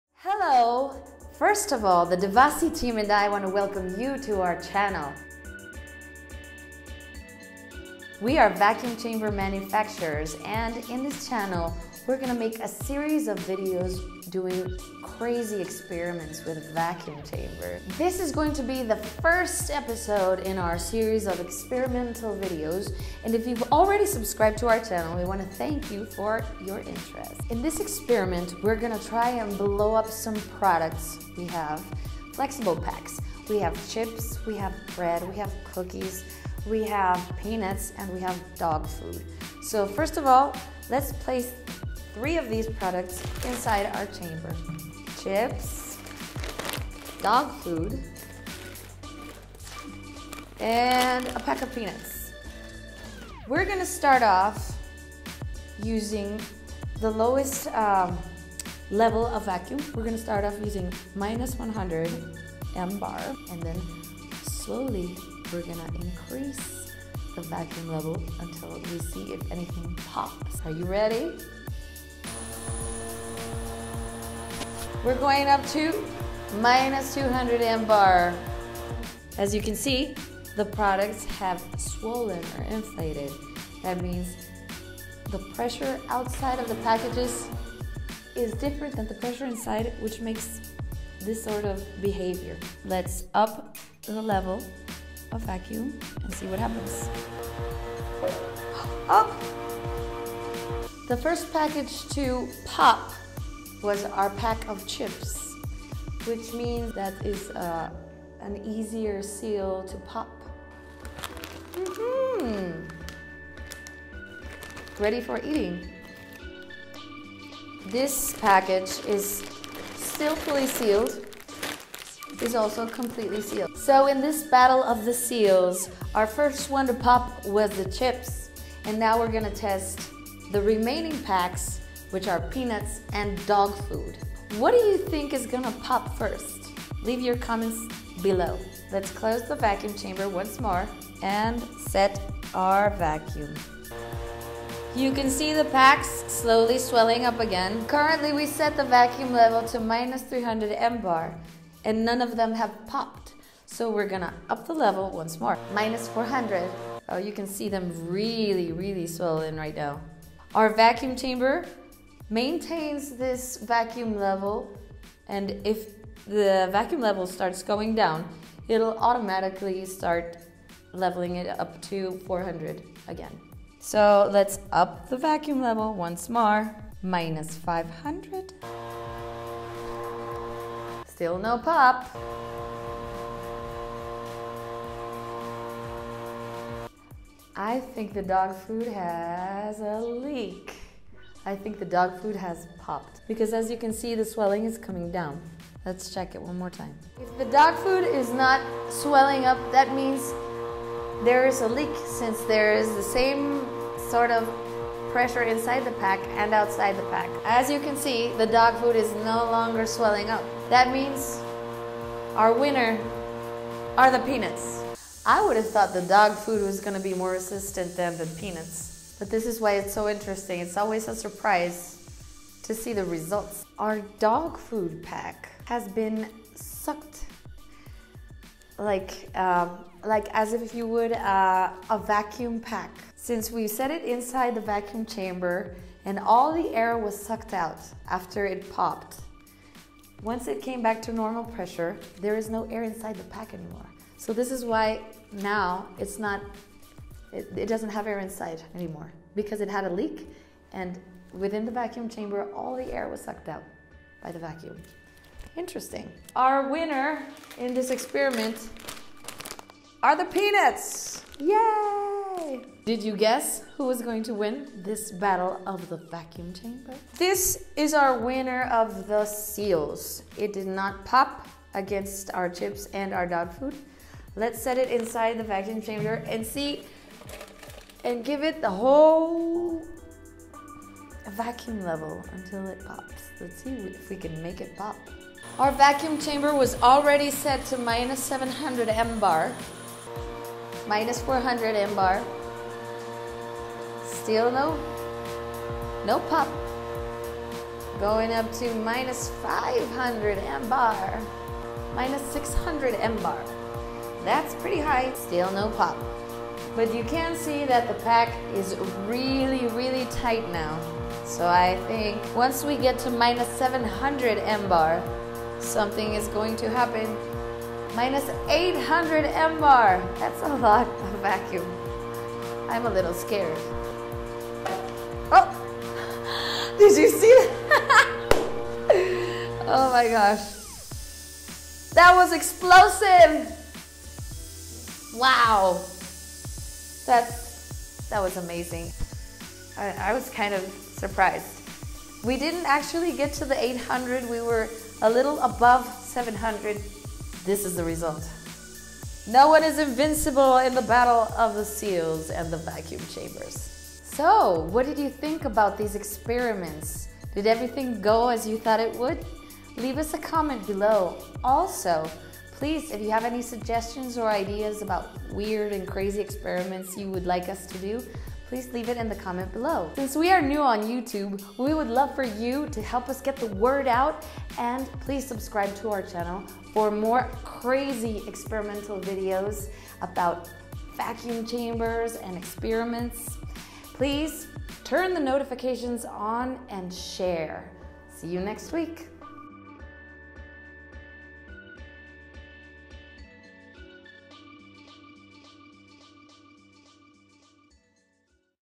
Battle of the Packages in Vacuum Chamber - Experiment Series - Episode #1
Vacuum Chamber Experiment- Episode 01
Testing seal strength and bursting packages in a vacuum chamber
This is a test in a vacuum chamber where you can see 3 different flexible packaging bags:
1- A bag of chips
2- Dog food Packaging
3- A bag of peanuts
We are doing to show you which one is the winner of the vacuum chamber bursting battle .
We are going to apply an extreme vacuum level at -800 mBar. As we are at 1800m above see level (Atmosphere pressure: 820 mbar), it will be almost absolute emptiness in our vacuum chamber!

We are an OEM Manufacturer of Vacuum Chamber CDV Model.